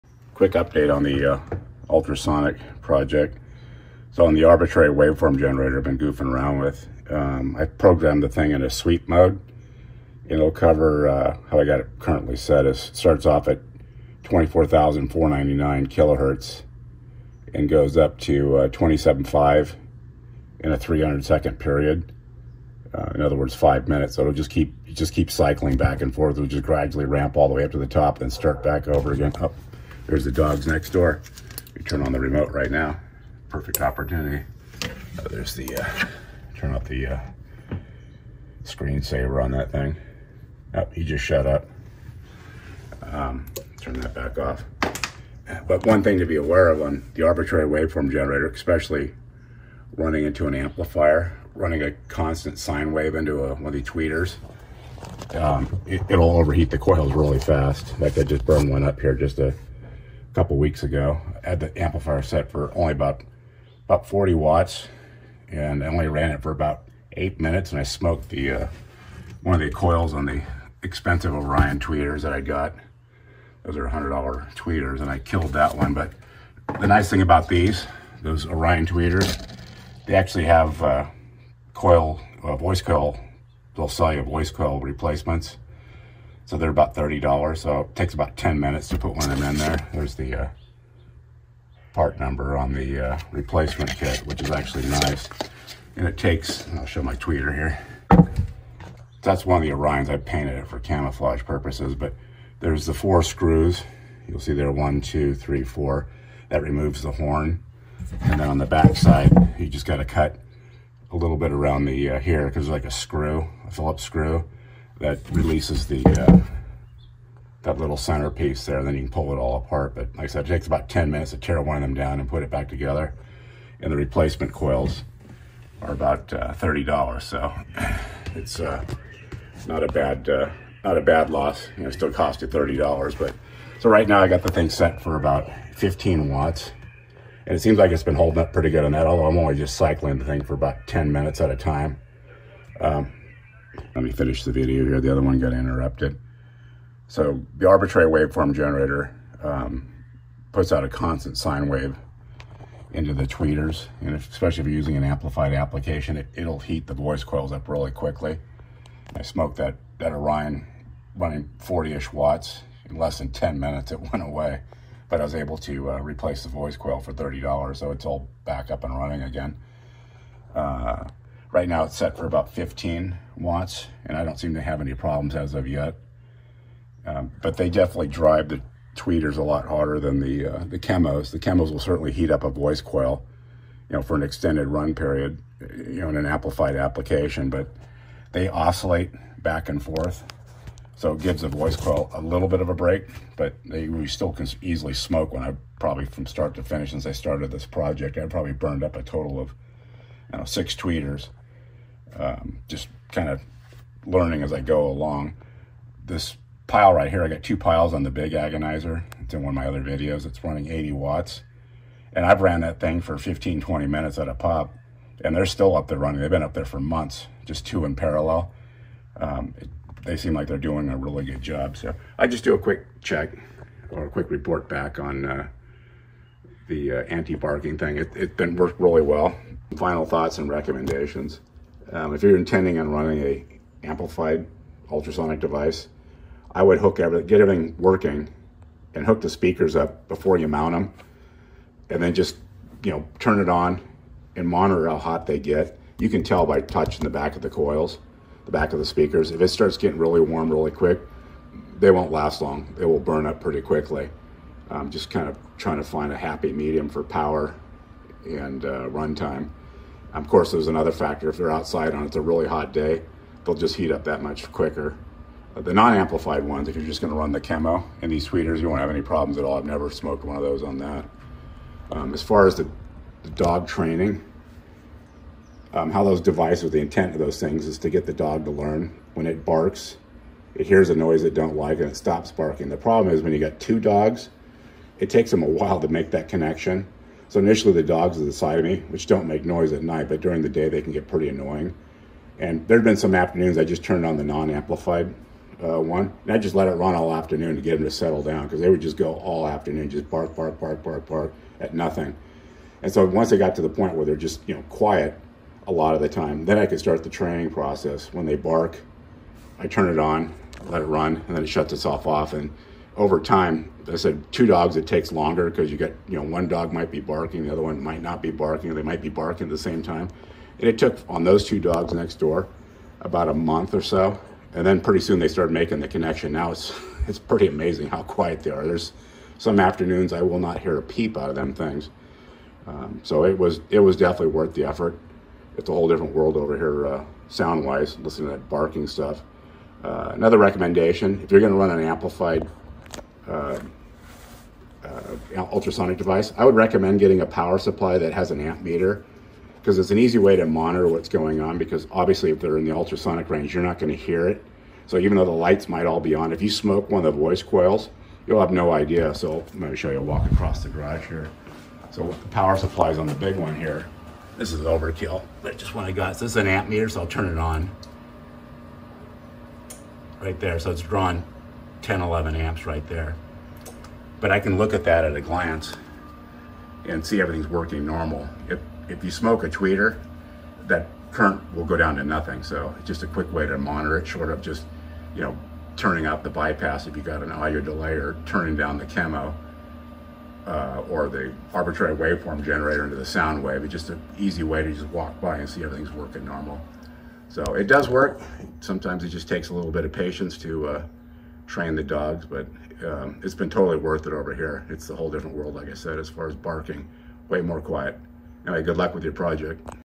Ultrasonic anti dog bark device update
Ultrasonic anti dog barking device, if your finding this video first see below video and it will make more sense.